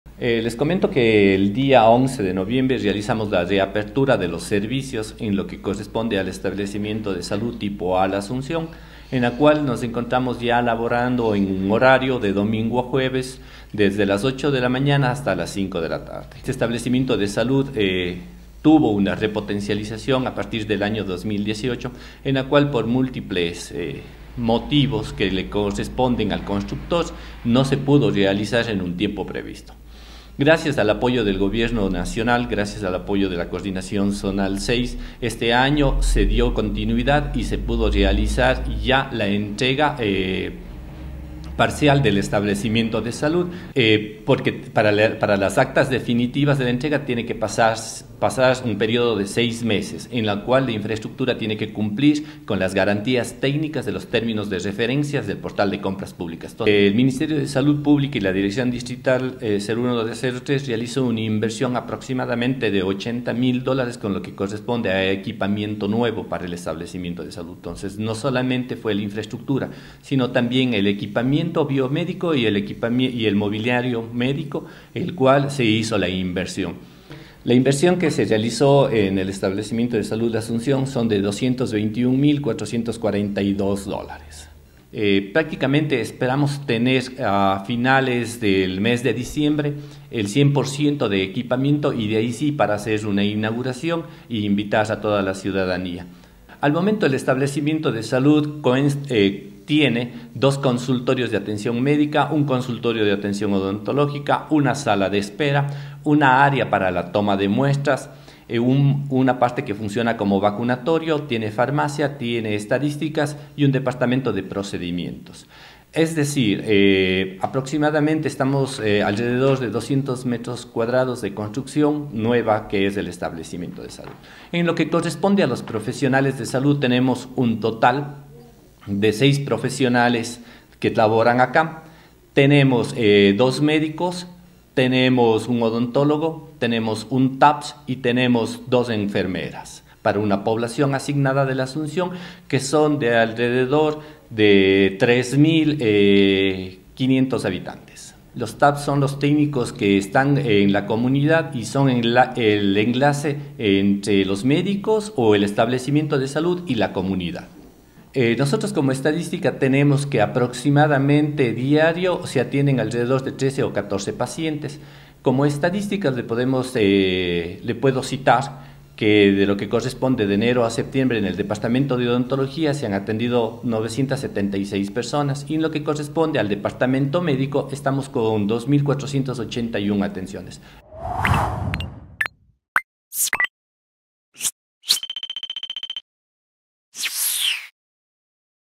En diciembre espera inaugurar repotenciado centro de salud de La Asunción
En la parroquia La Asunción, los trabajos de readecuación del centro de salud han concluido con una importante inversión desde el estado central, dio a conocer el director distrital de salud 01D03 de Girón a Santa Isabel. A finales del año se podría inaugurar la obra, pero la recepción definitiva deberá esperar el tiempo reglamentario.